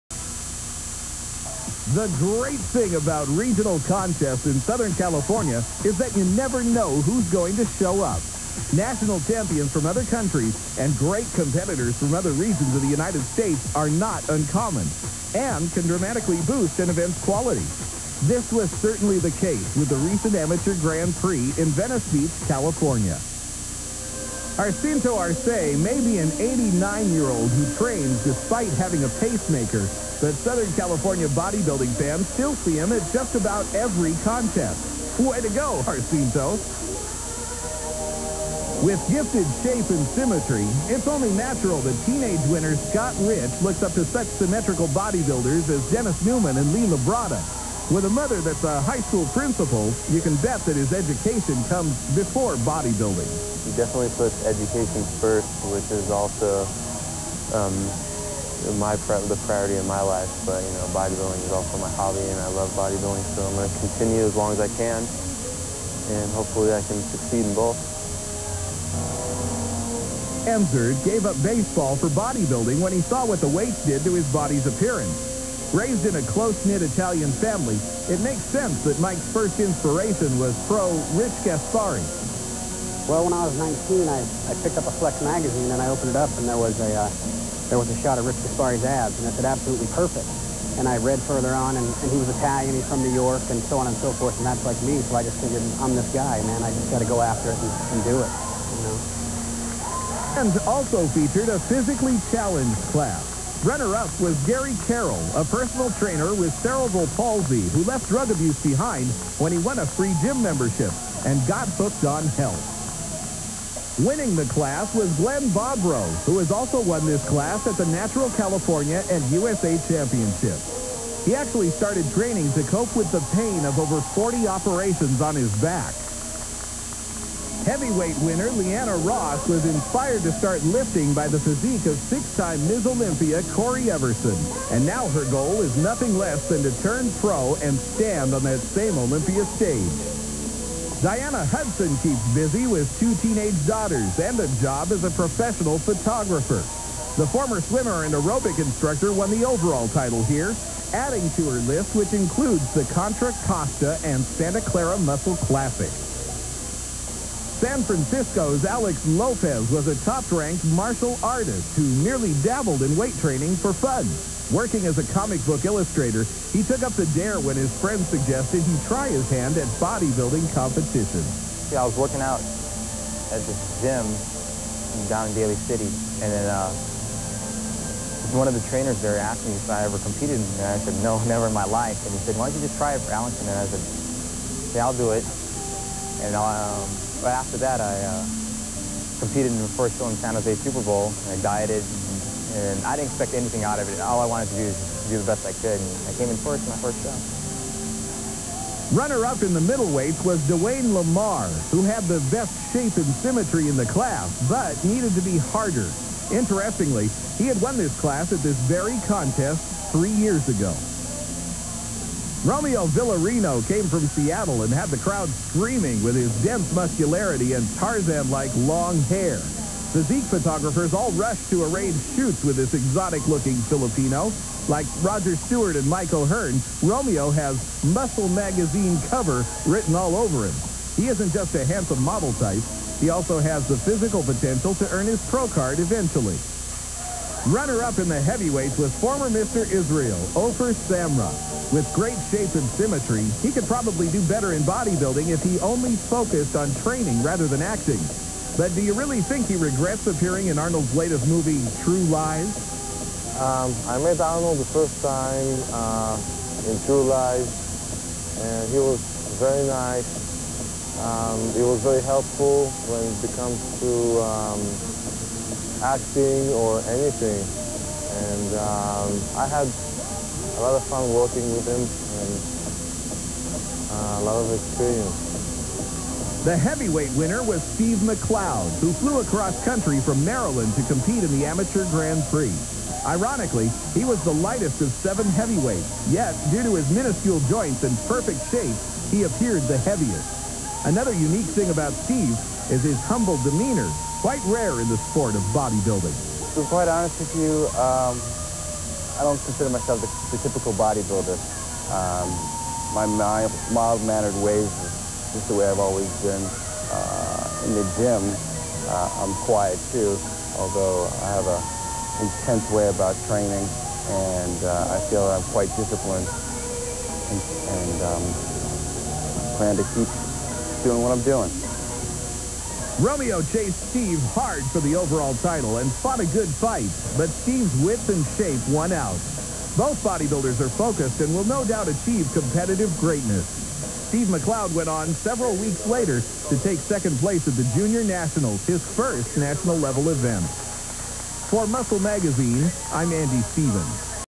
1994 NPC Amateur Grand Prix Bodybuilding Championships - Masters, Disabled, Teen, Women, Men
Great 1990s Show:
Scott Rich Teen Champ
Diana Hudson Women's Champ
Notable Men: Romeo Villarino, Ofer Samra, Steve McLeod